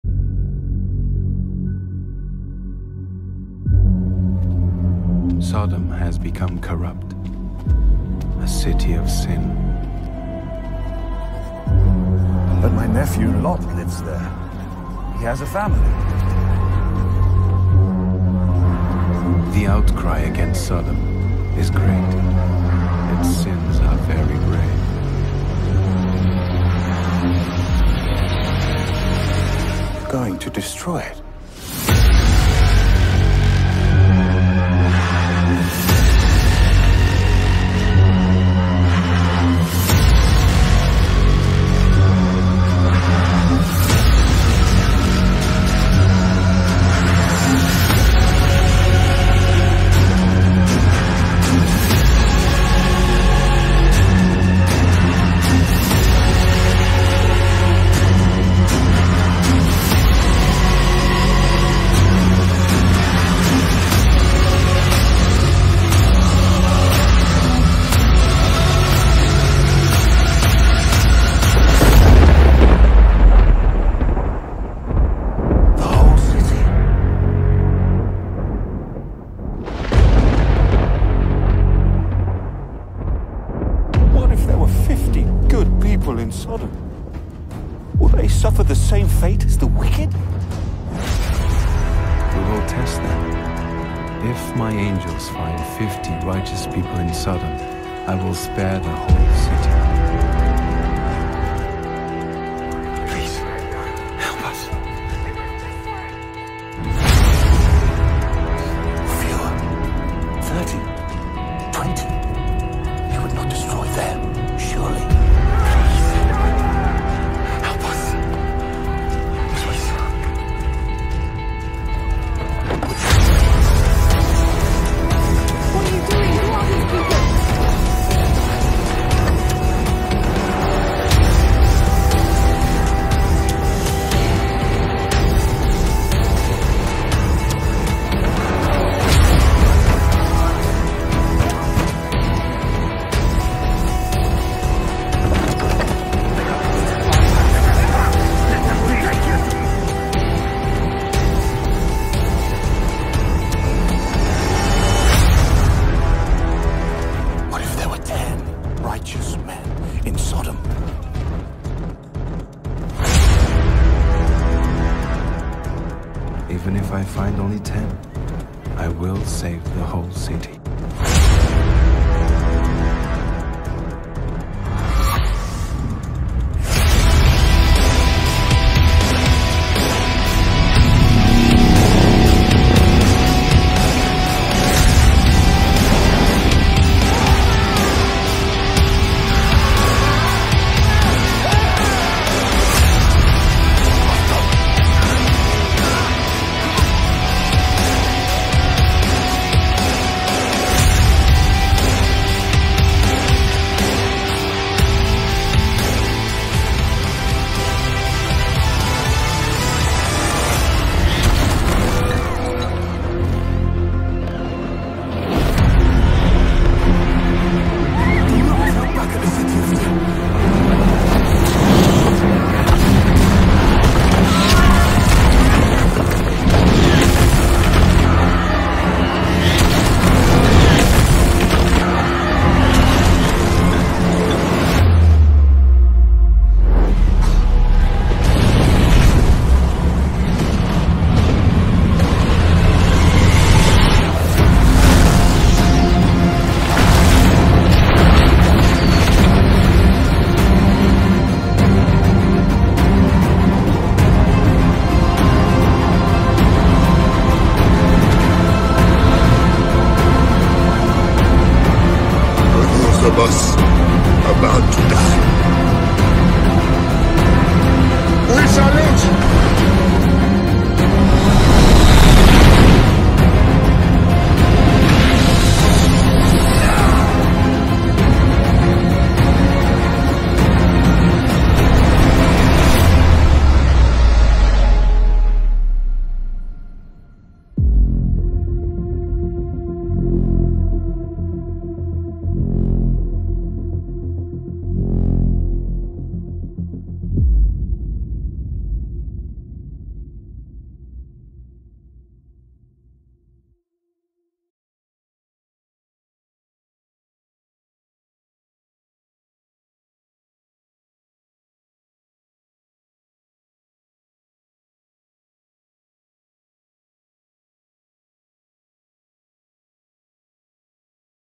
THE BIBLE SERIES: PART 8 (SODOM & GOMORRAH)
'SODOM & GOMORRAH' is an epic visual of the destruction of the cities of Sodom & Gomorrah